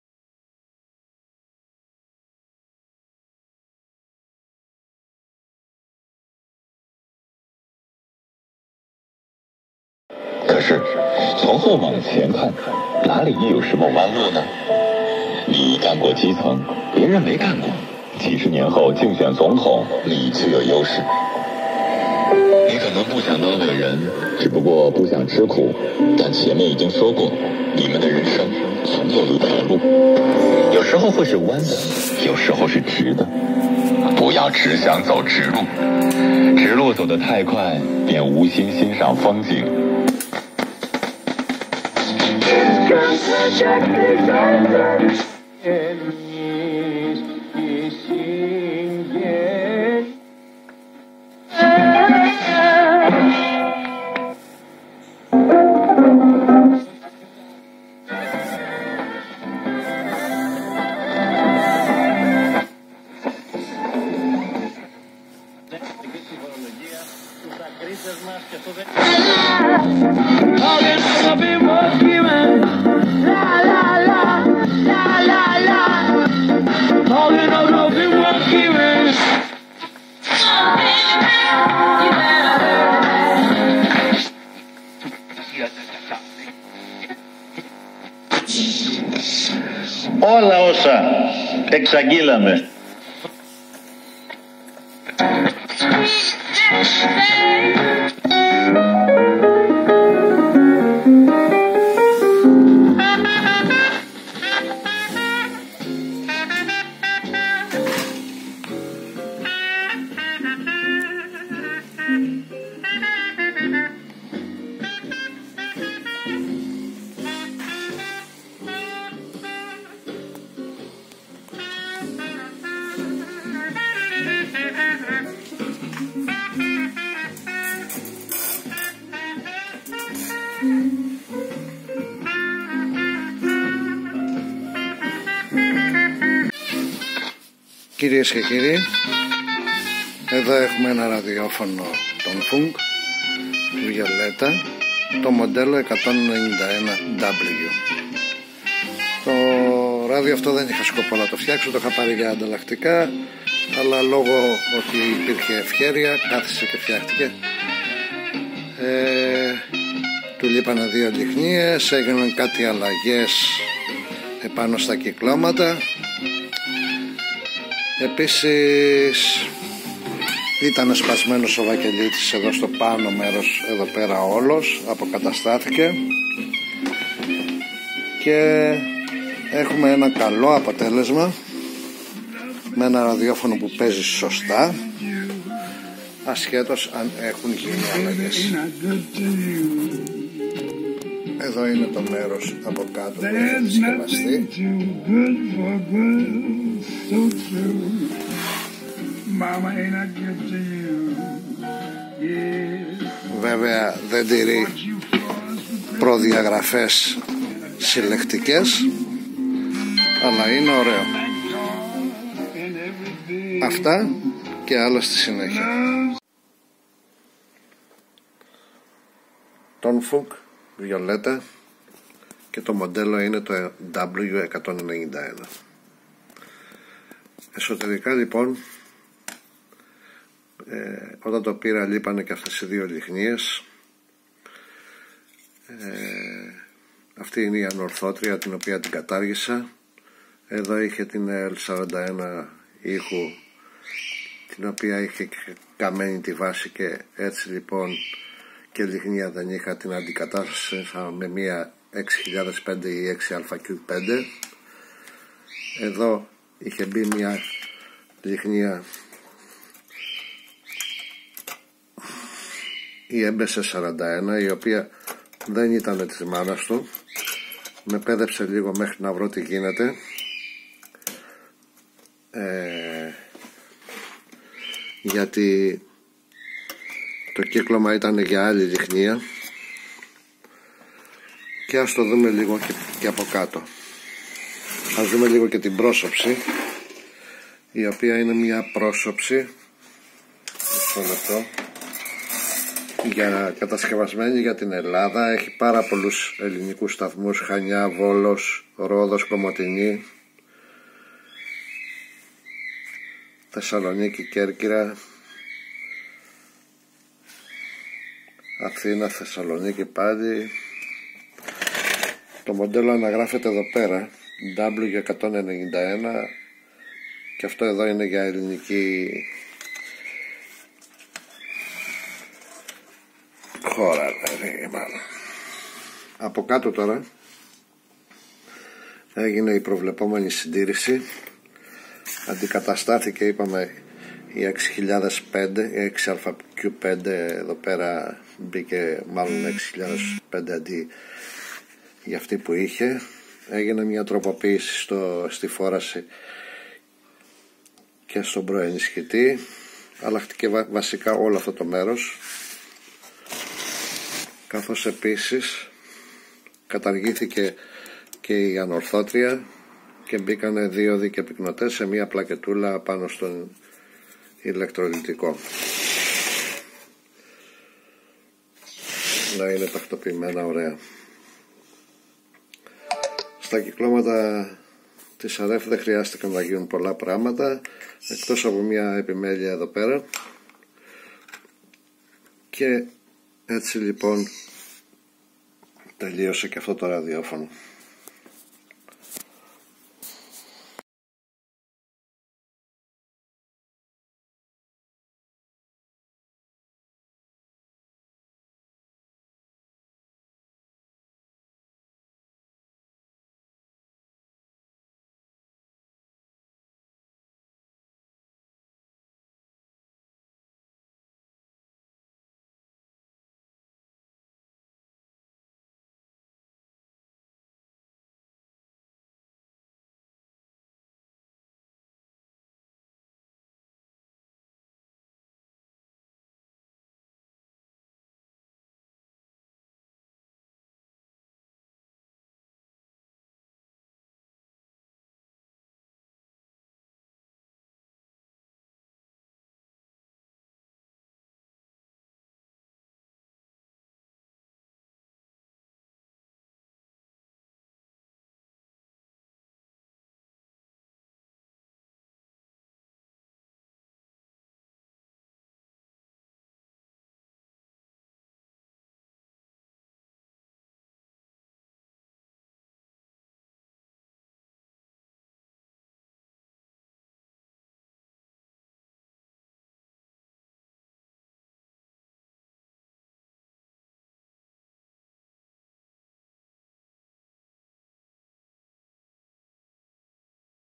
TONFUNK VIOLETTA W191 (1951)
Θα δείτε την αποκατάσταση του πλαισίου και τις αλλαγές των κυκλωματων που γίνανε σε αυτόν τον δέκτη.